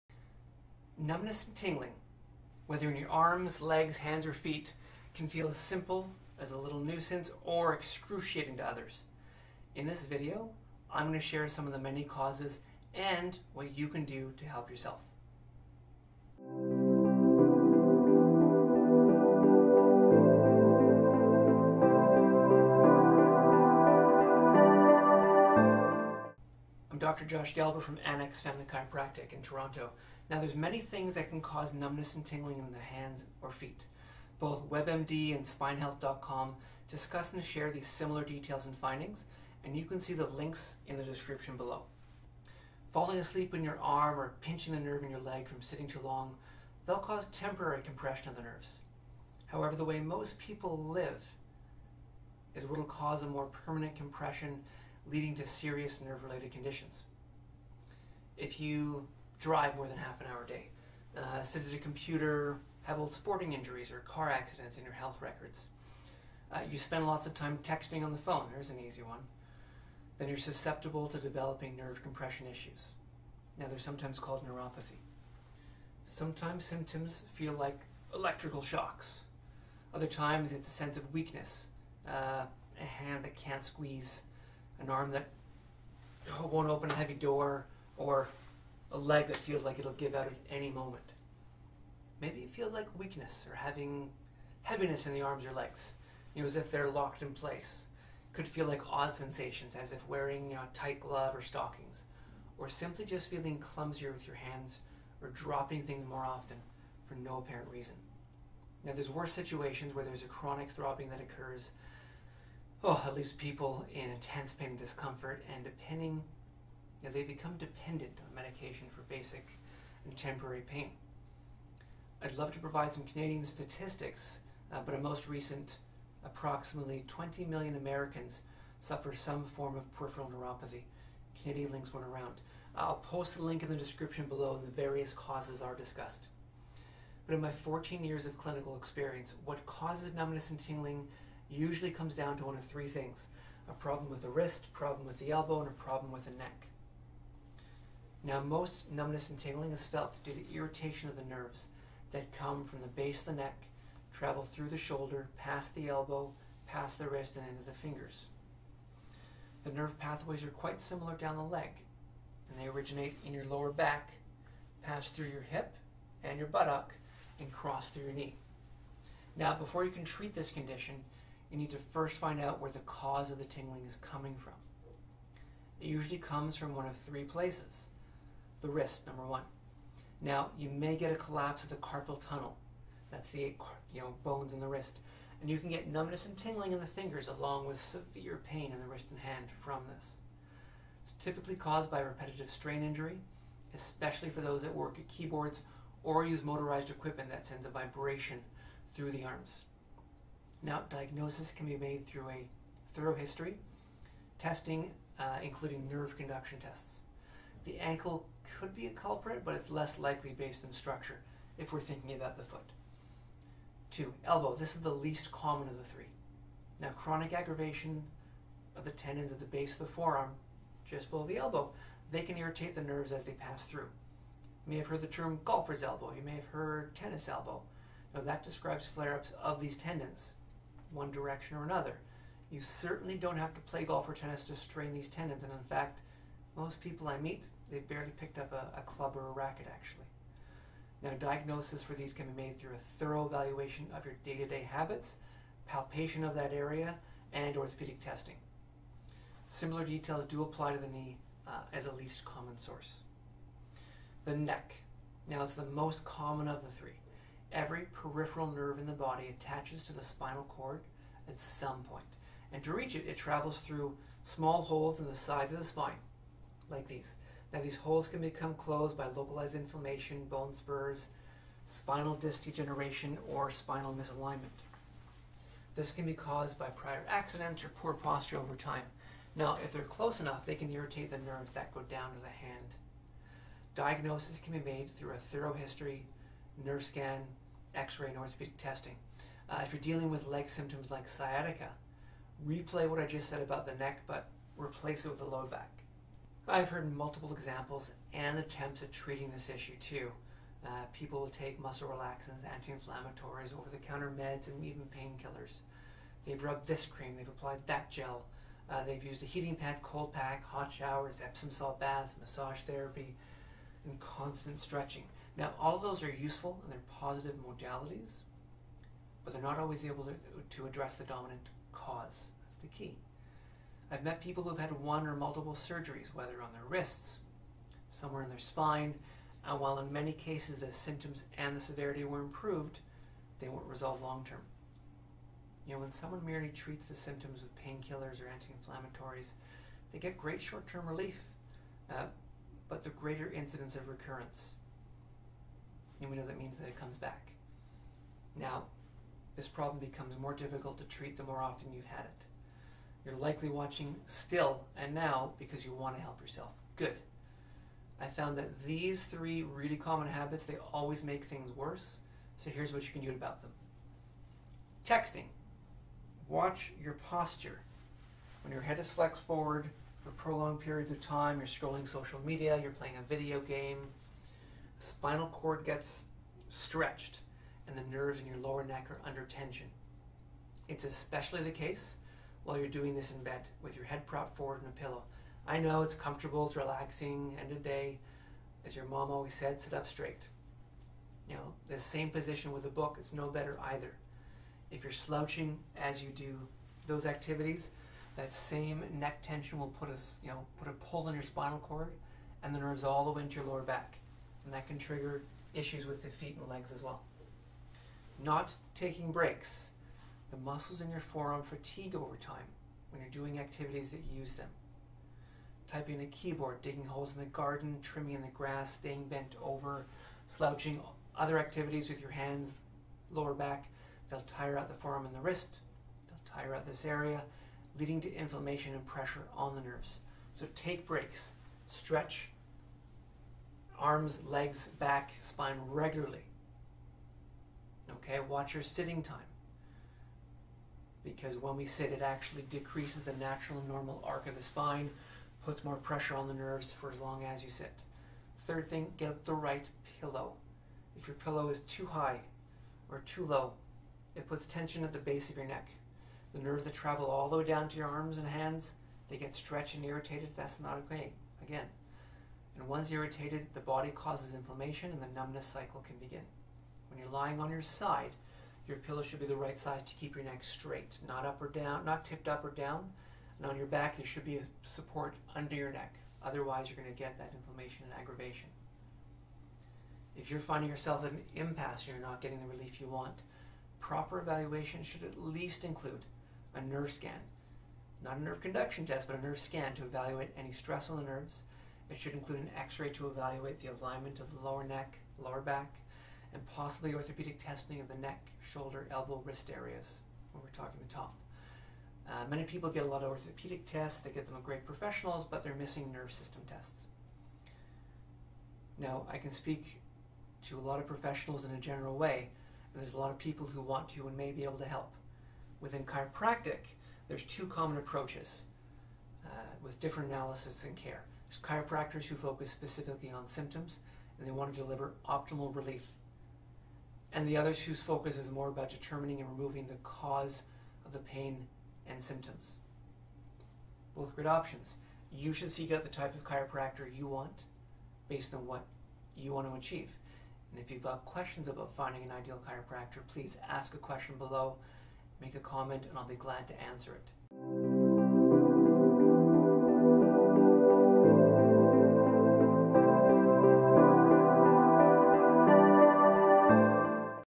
Numbness or Tingling: Top 3 “A-HA” Solutions
There are many things that can cause numbness and tingling in the hands or feet; both WebMD and Spine-Health discuss and share these similar details and findings.  Falling asleep on your arm or pinching the nerve in your leg from sitting too long cause temporary compression of the nerves, however the way most people live cause can cause a more permanent compression leading to serious nerve related conditions.  If you drive more than half an hour a day, sit at a computer, have old sporting injuries, car accidents or spend lots of time texting on the phone then you are susceptible to developing nerve compression issues (sometimes called neuropathy)

3 Peer-reviewed studies and their summary statements regarding chiropractic care:
---Journal of Chiropractic Medicine 2008: Patients showed an 85.5% resolution of the nerve symptoms after only 9 chiropractic treatments.
---JMPT 1998: With chiropractic care, patients had “significant improvement in perceived comfort and function, nerve conduction and finger sensation overall.”
---JMPT 1994: “Significant increase in grip strength and normalization of motor and sensory latencies were noted. Orthopedic tests were negative. Symptoms dissipated.”